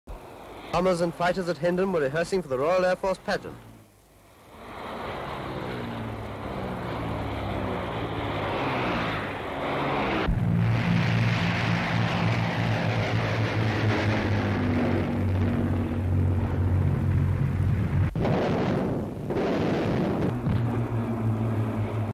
Aeroplanes practice at Hendon for R.A.F. pageant (1936)
Royal Air Force planes practice for an air display at Hendon

Full Description:

SLATE INFORMATION: From Hendon

ENGLAND: London: Barnet: Hendon:
EXT

AEROPLANES, R.A.F. Rehearsal for R.A.F. Pageant at Hendon

HENDON 'drome (AIR DISPLAY) R.A.F. rehearse for Air Pageant

Airforce - Active; Royal Air Force

aviation, aeroplane, air show, aviators, explosion, RAF, aeroplanes, airplane, airplanes, plane, planes
Background: Royal Air Force planes practice for an air display at Hendon